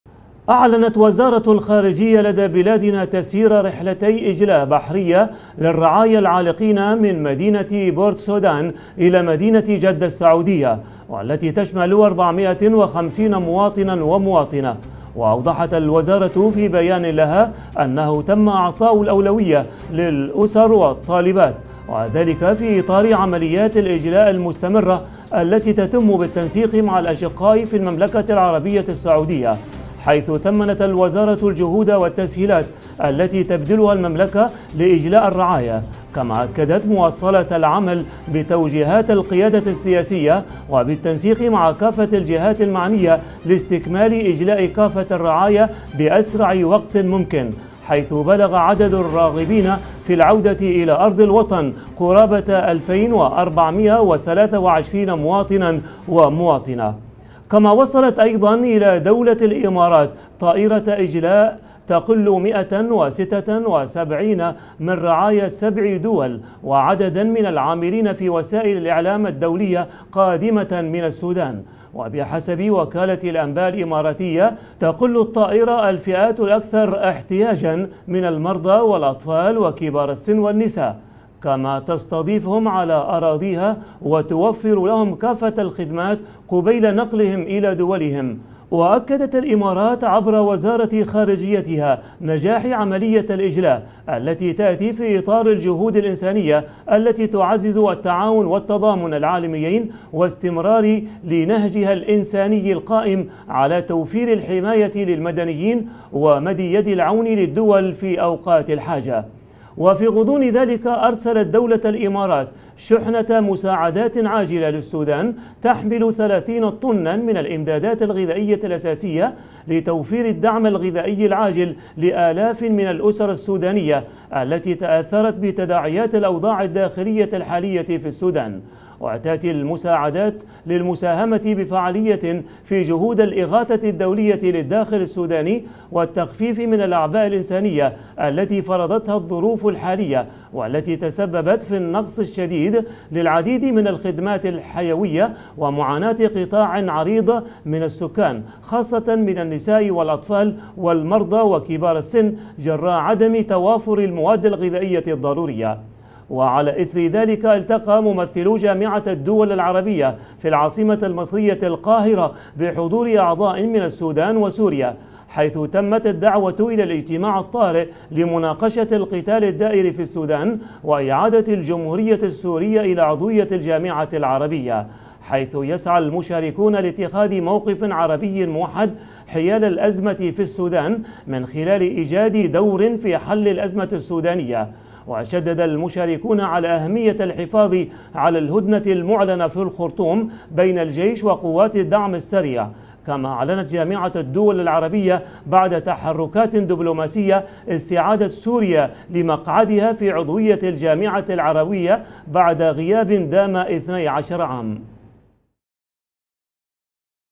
المحور_الدولي | إجلاء 450 من رعايا بلادنا إلى جدة.. والجامعة العربية تبحث الاستقرار في السودان
تجلي بلادنا أربعمائة وخمسين مواطناً كدفعة أولى من أصل ألفين وأربعمائة وثلاثة وعشرين مواطناً طلبوا إجلاءهم إلى مدينة جدة كما تتواصل أيضاً عمليات الإجلاء وتقديم المساعدات التي تقوم بها دولة الإمارات وجامعة الدول العربية تجتمع لبحث الاستقرار في السودان